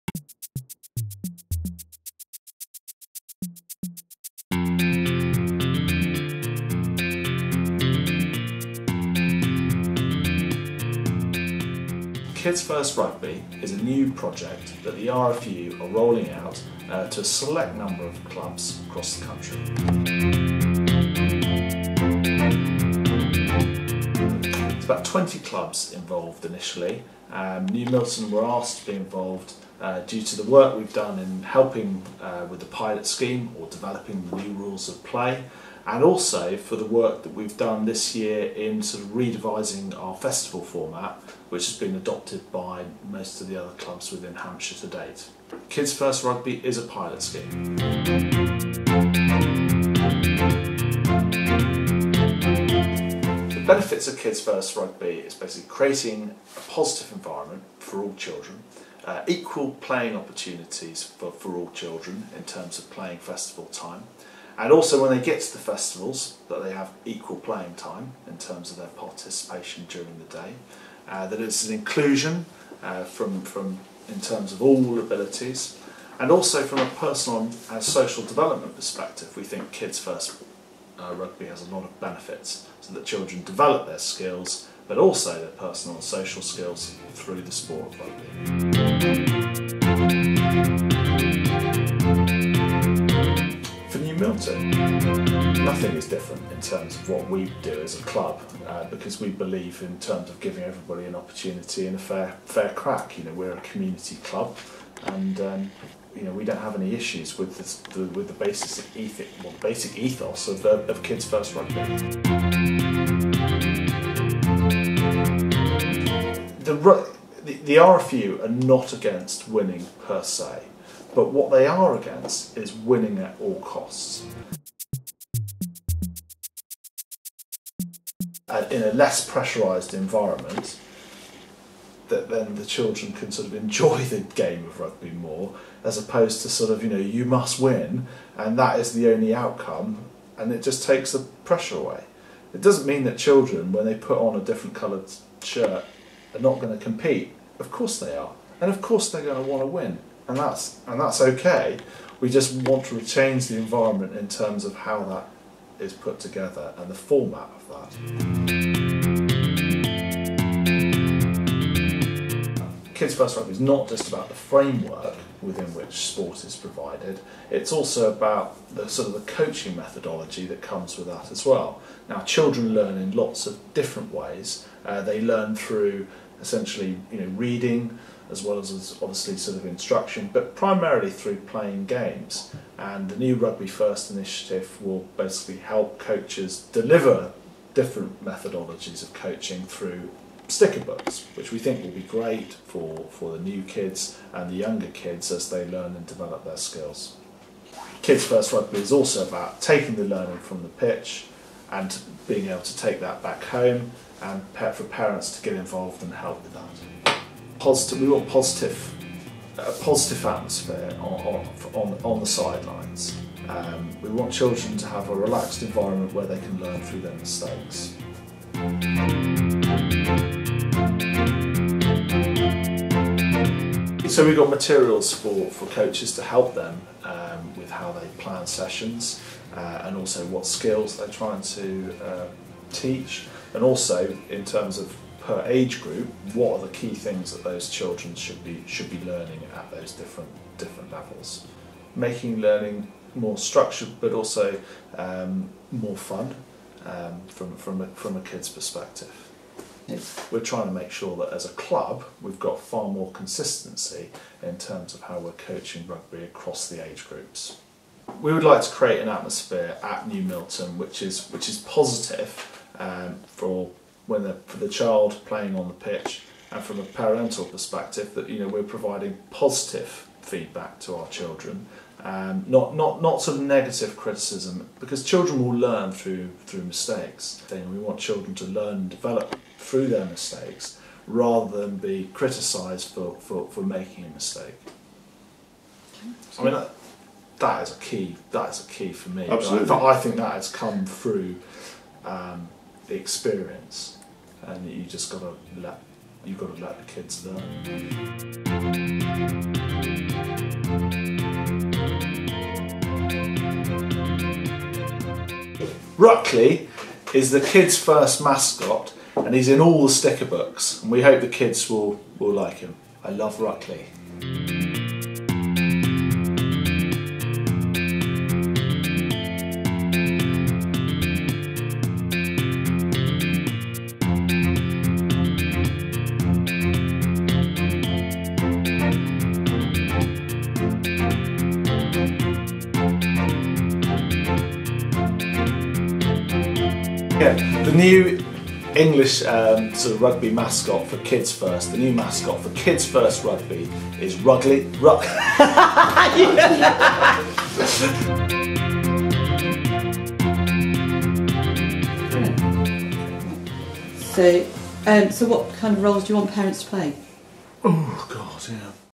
New Milton Kids First Rugby - Briefing for Parents
New Milton Minis Rugby are part of teh Kids First Rugby pilot scheme being rolled out by the RFU.
This short film is a briefing for parents to answer some of the obvious questions.

This is a pilot scheme, the RFU and New Milton would like all parents and coaches to give it a real go, but also value feedback to help make it even better